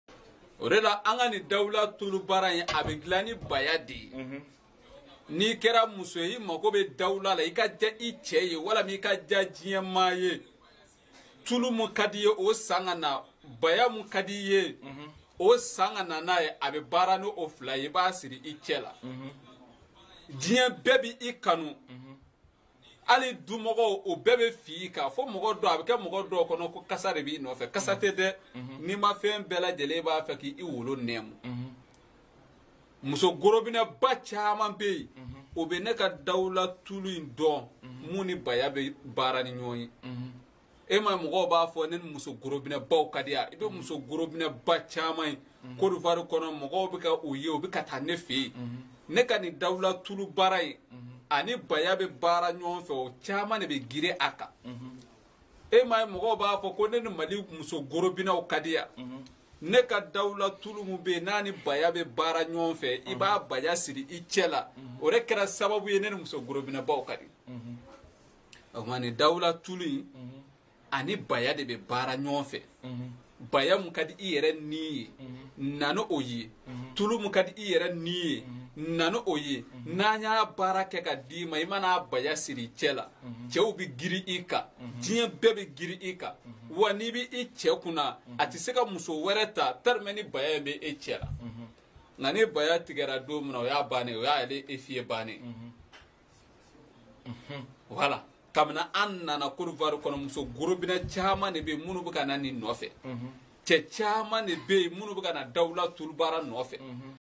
karamoko befo junior international les femmes qui veulent que leur mari soit a elles seuls
Mohamed Bachir kissa dit junior international, guide de l'association Bachirou -Dine
Plus d'informations appeler sur whasap